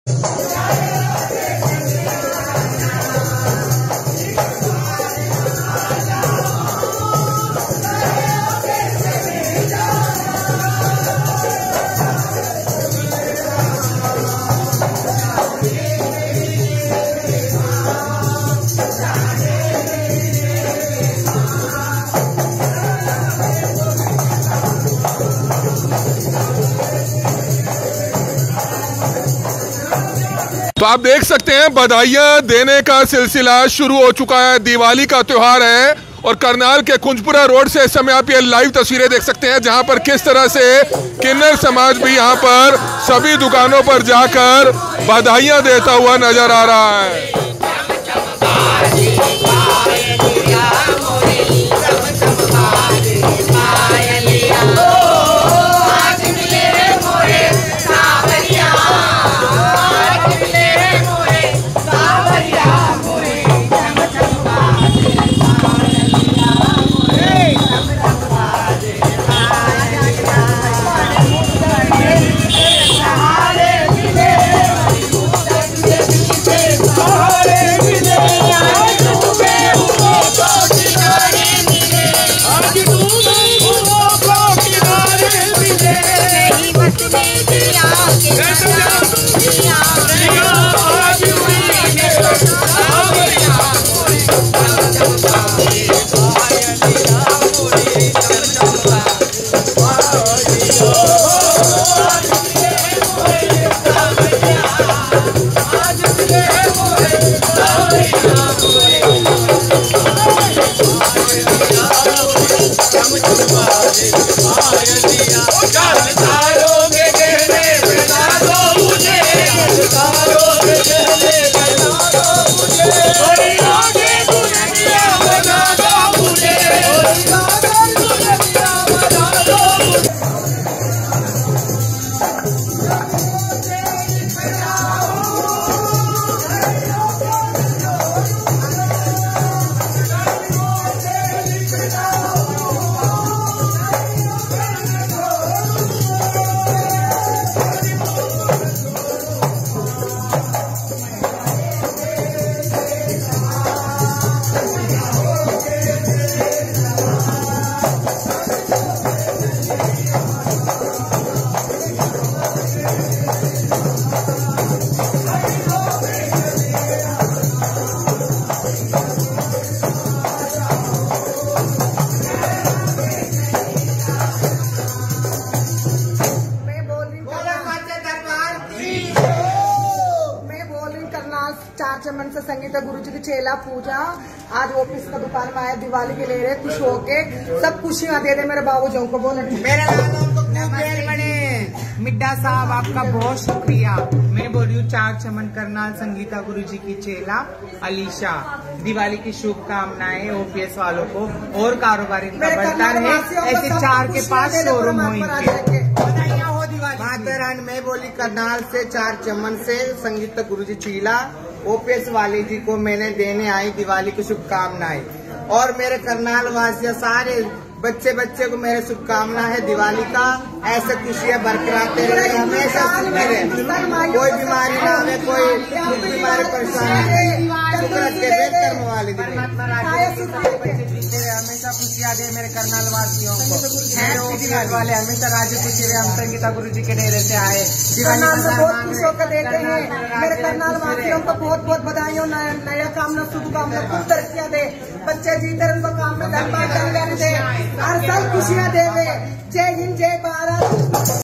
किन्नर समाज पहुंचा करनाल के बाजारों में, दिवाली की शुभकामनाएं देते हुए जमकर किया डांस
किन्नर समाज पहुंचा करनाल के बाजारों में, दिवाली की शुभकामनाएं देते हुए जमकर किया डांस, देखें Live